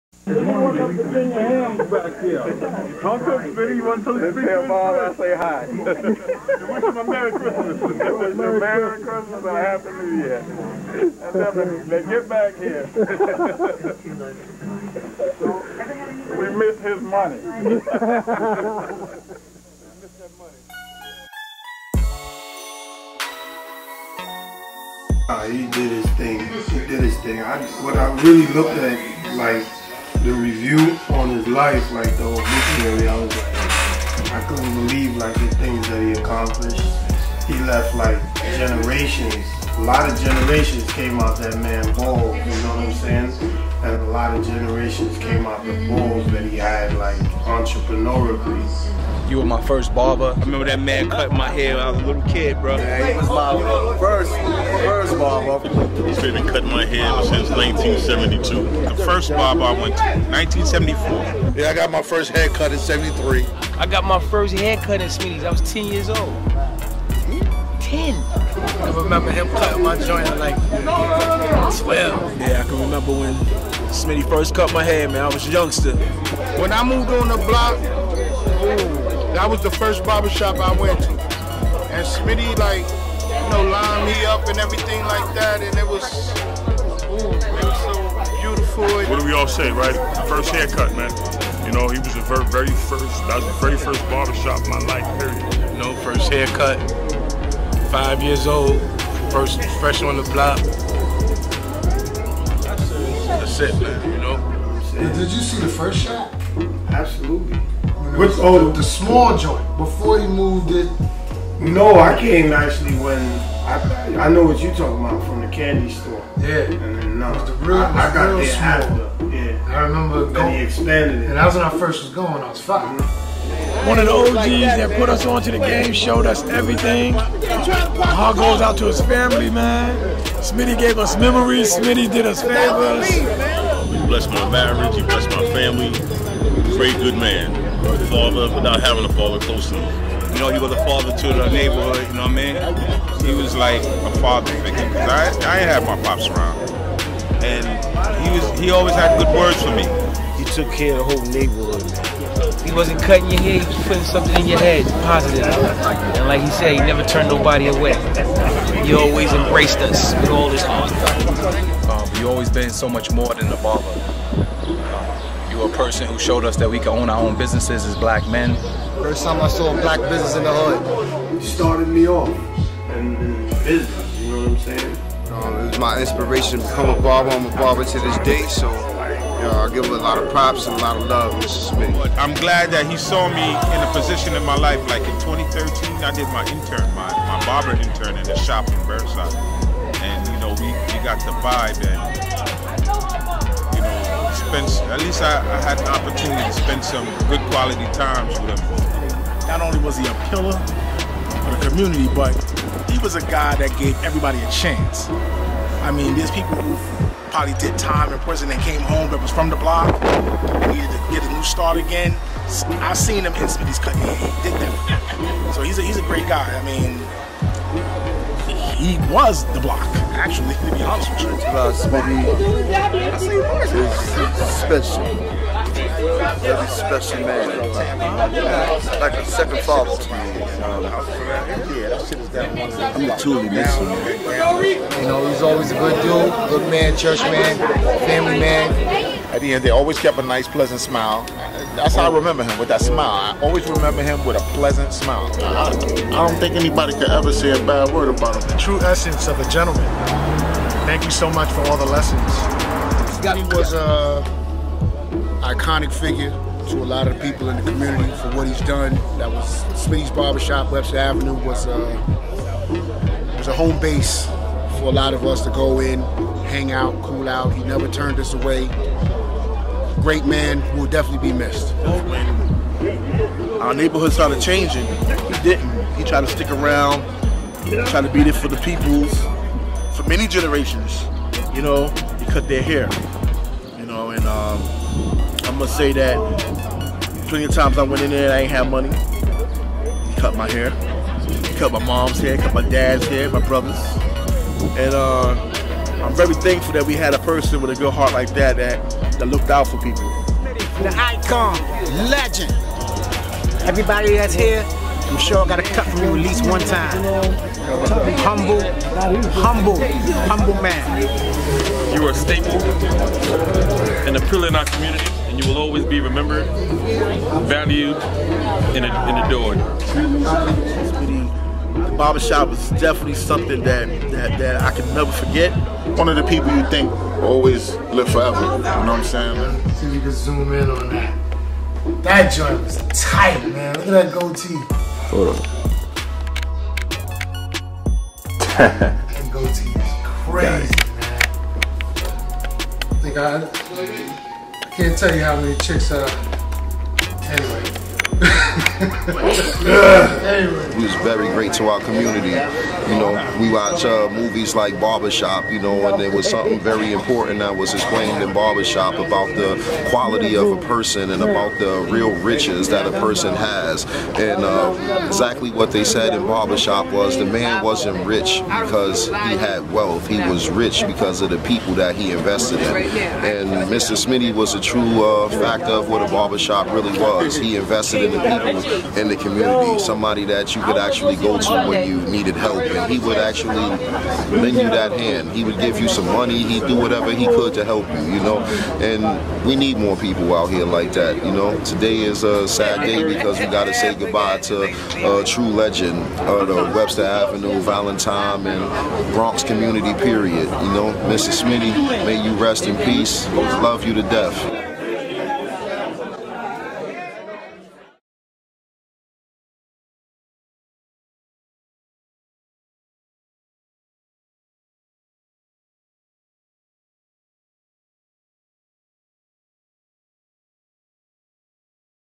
Mr. Smitty's Barbershop tribute
A host of friends, family and clients of Mr. Smitty tell stories of the impact he had on their lives.